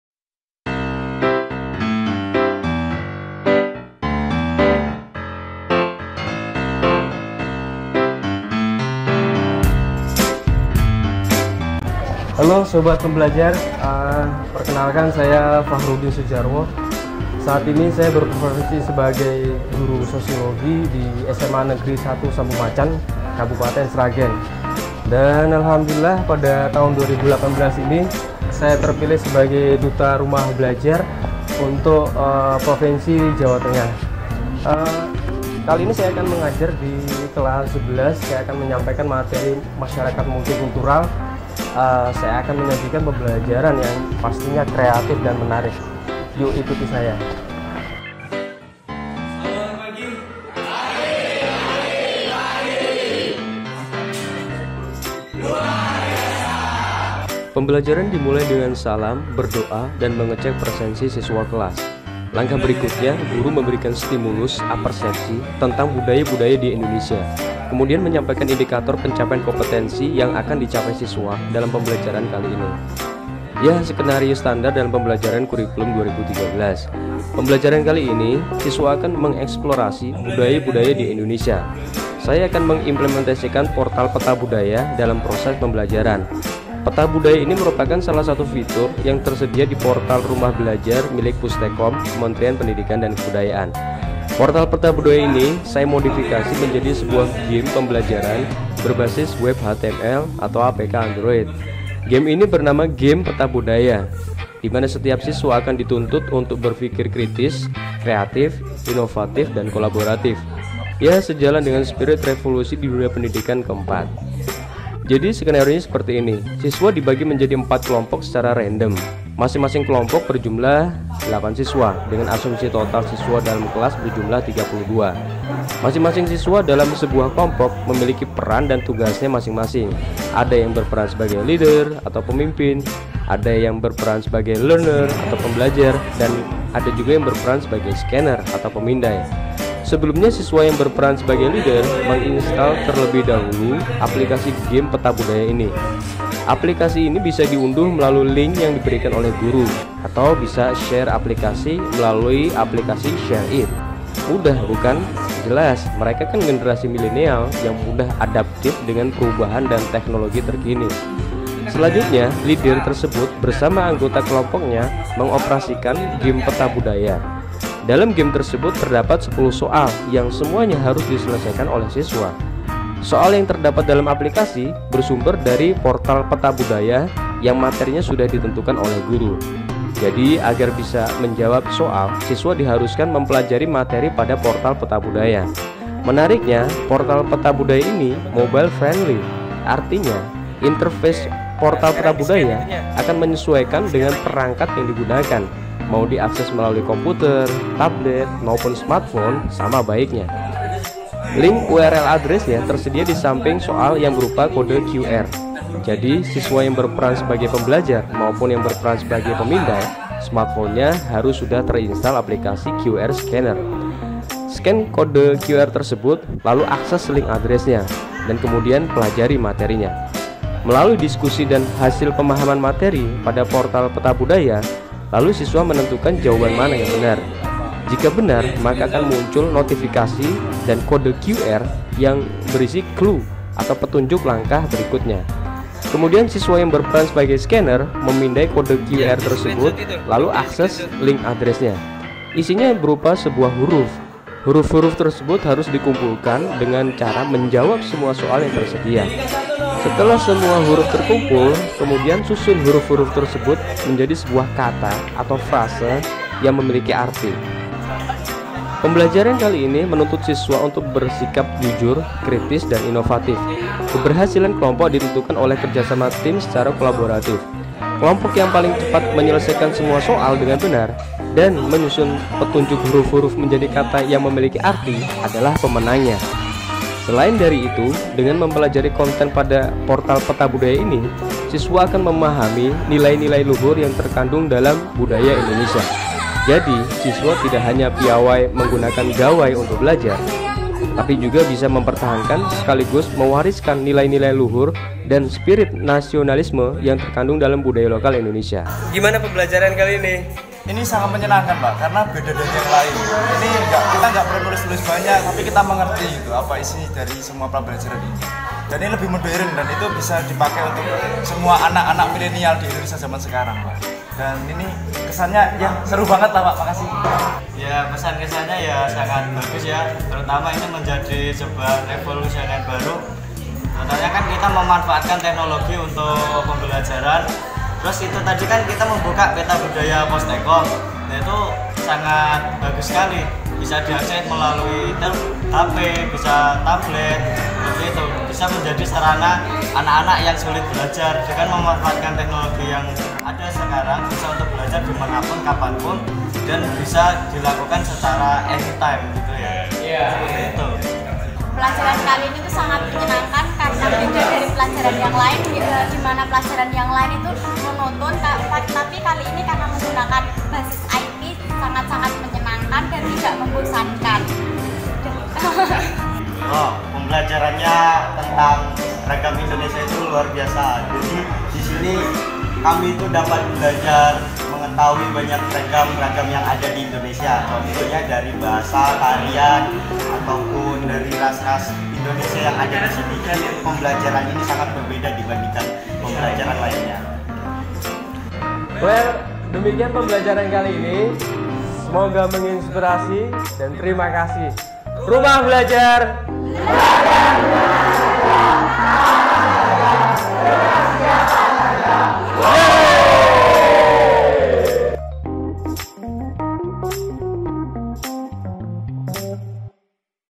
Pembelajaran Sosiologi Kreatif Ala Duta Rumah Belajar
Implementasi portal Rumah Belajar dalam proses pembelajaran berbasis TIK. Inspired by Jerry Wickson. Thanks a lot.